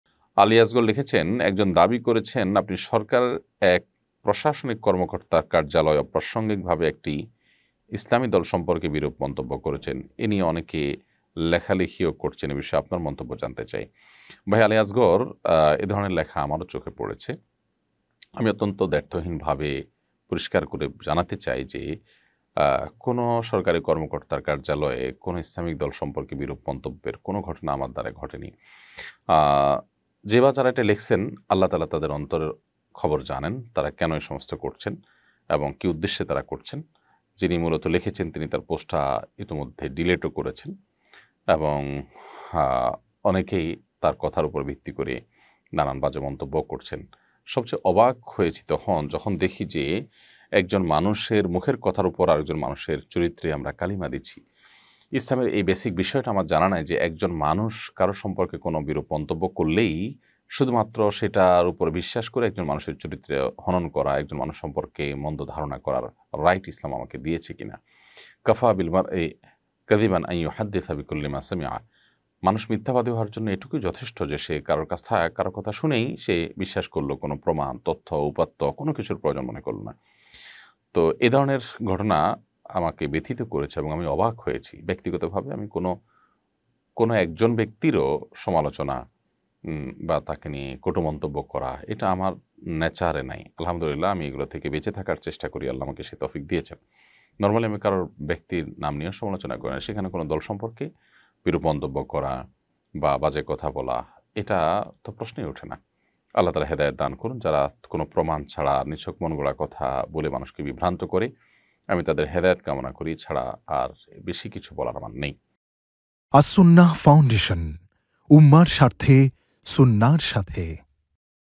নিজের বিরুদ্ধে মিথ্যা অপবাদমূলক পোস্টের জবাবে যা বললেন শায়খ আহমাদুল্লাহ
নিজের বিরুদ্ধে মিথ্যা অপবাদমূলক পোস্টের জবাবে যা বললেন শায়খ আহমাদুল্লাহ new waz II waj II Sheikh ahmadullah